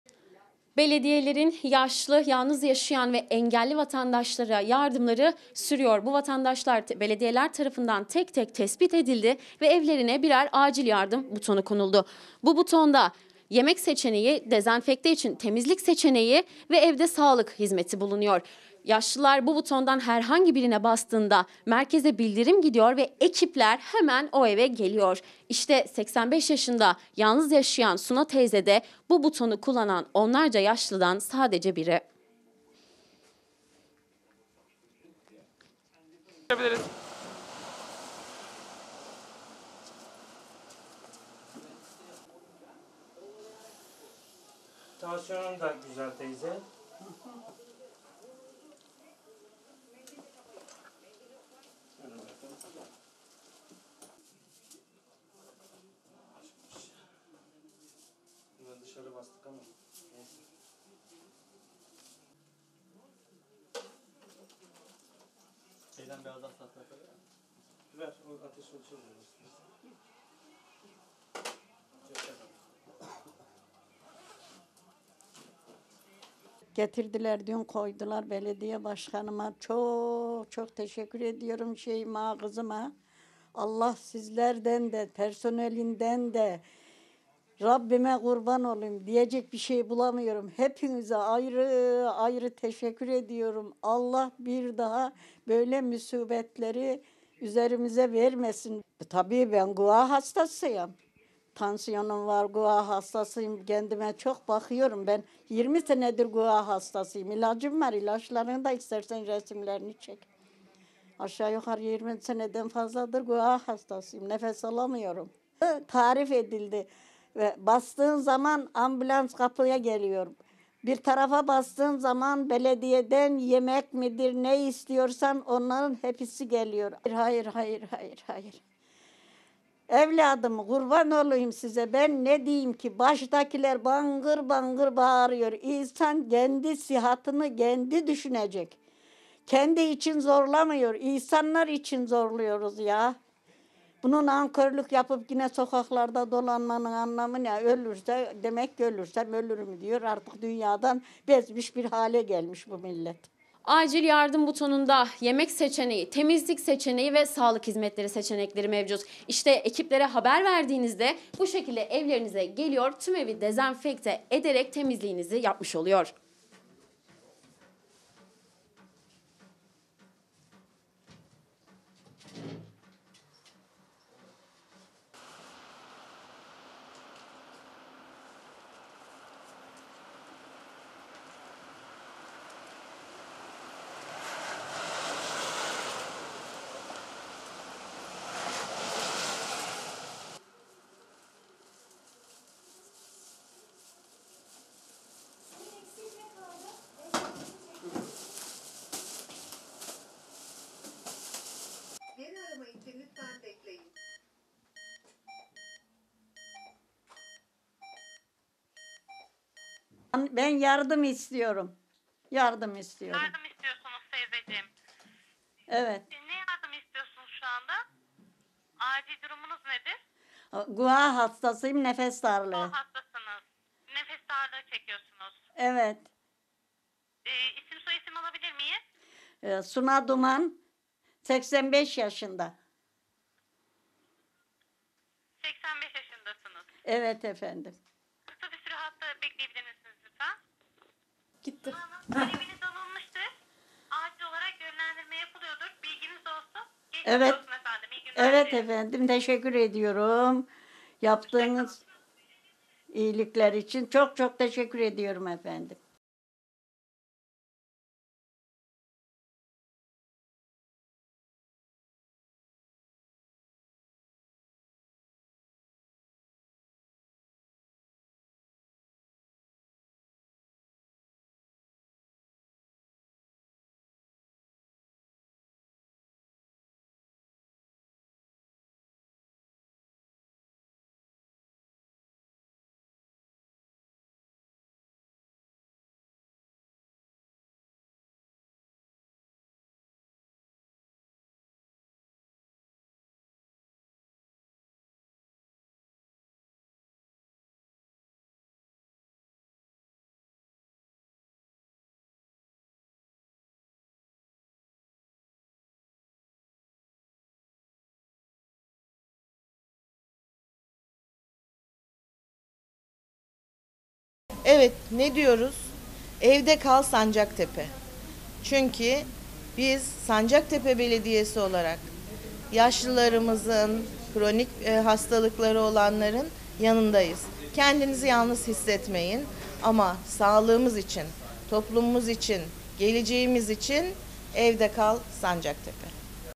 Sancaktepe’de acil yardım butonu devrede
Sancaktepe Belediyesi ilçede yaşayan engelli, yatağa bağımlı ve 60 yaş üstü vatandaşlara hizmet verecek 'Acil panik butonu' sistemini hayata geçirdi. Amaç vatandaşların her türlü hizmeti evlerinden çıkmadan alabilmesini sağlamak oldu. 85 yaşındaki yalnız yaşayan ve koah hastası Suna Duman, “Bu buton çok iyi oldu. Düşünenden bulandan Allah bin kere razı olsun” dedi.

Sancaktepe Belediyesi ilçe genelindeki tüm yaşlı, engelli ve yalnız yaşayan vatandaşların evine acil yardım butonu yerleştirdi. Butonda üç çeşit hizmet bulunuyor. Herhangi bir durumda vatandaşlar evlerine kurulan sistem sayesinde çağrı merkezine ve sisteme entegre edilmiş sağlık ekibine ulaşabilecek. Eve doktor ve acil tıp teknisyeninin bulunduğu ambulans yönlendirilecek. Vatandaşların kişisel ihtiyaçları da çağrı merkezi tarafından dışarıdaki belediye ekiplerine iletilerek giderilecek.

“TEK BUTONLA EVLERE SAĞLIK EKİPLERİ GİDECEK”

Sancaktepe Belediye Başkanı Av. Şeyma Döğücü, Sancaktepe’de 65 yaş ve üstü, engelli ve yatağa bağımlı vatandaşların, koronavirüs (COVID-19) şüphesi ve acil durumlarda basabileceği panik butonu devreye girdiğini belirterek “Vatandaşlarımız herhangi bir durumda evlerine kurulan sistem sayesinde çağrı merkezine ve sisteme entegre edilmiş sağlık ekibine ulaşacak. Eve doktor ve acil tıp teknisyeninin bulunduğu ambulans yönlendirilecek. Vatandaşların kişisel ihtiyaçları da çağrı merkezi tarafından dışarıdaki belediye ekiplerine iletilerek giderilecek” şeklinde konuştu.

“BEN ÇIKMIYORUM NE OLUR SİZ DE SOKAĞA ÇIKMAYIN”

Sancaktepe’de yalnız yaşayan 85 yaşındaki Suna Duman'ın da evine acil yardım butonu yerleştirildi. Verilen hizmetten çok memnun olduğunu belirten koah hastası Duman, “Belediyemize personelinden hizmetlisine kadar, bizi düşünen kim varsa hepsine çok teşekkür ediyorum. Allah bu millete bir daha böyle musibetler yaşatmasın. Benim tansiyonum var ve 20 yıldır koah hastasıyım. Bize bu butonları verdiler. Tarif ettiler. Bastığın zaman ambulans kapıya geliyormuş. Bir diğerine bastığında yemek getiriyorlar. Ne istersek geliyor. Bizler için çok iyi oldu” dedi. Hala dışarıya çıkarak hem kendini hem başkalarını riske atanlar için ise, “Evladım size kurban olurum ne olur yalvarıyorum kapıya çıkmayın. Herkes üstüne basa basa söylüyor. İnsan kendi sıhhatini kendi düşünecek” ifadelerini kullandı.

Öte yandan Sancaktepe'deki tüm kamu kurum ve kuruluşları ile okullar, camiler ve cemevleri dezenfekte edilirken vatandaşlara içinde sabun, ıslak mendil ve dezenfektan ürünlerinin bulunduğu çantalar dağıtıldı.